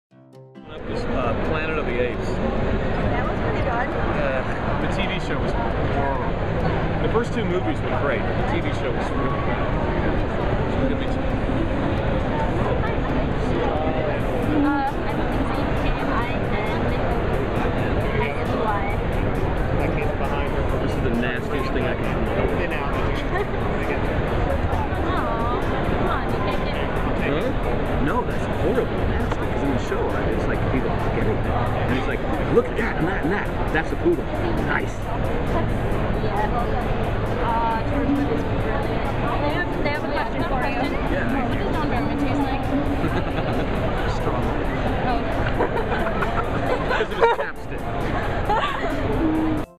What does John Barrowman taste like?
STRAWBERRIES
OMG
I lovez James Marsters
SPIKEEEEEE
CAPTAIN JOHHHHN

hahaaa... Kinz and I both received free strawberries in different places in Manhattan today. I THINK IT'S A SIGN.

this was filmed on October 9, 2010 at the NY Comic Con ;D